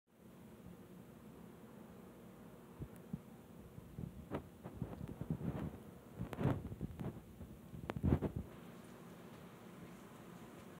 Frisky Attacks Camera
Frisky grabs the camera.

Frisky is an eleven month old gray tabby male kitten/cat. 7/23/2022